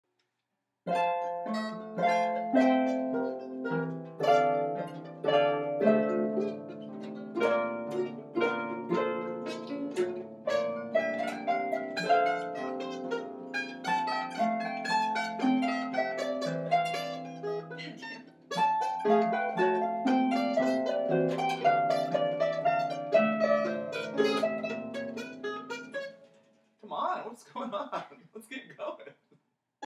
Harp Practice, Interrupted
Matthew attempts to accompany (distract) me on my super out of tune mandolin... Coming soon to a wedding near you! Or... near a wedding near you. Because no one sane would ever hire us so we'll be the random people playing out in the parking lot... being asked to leave...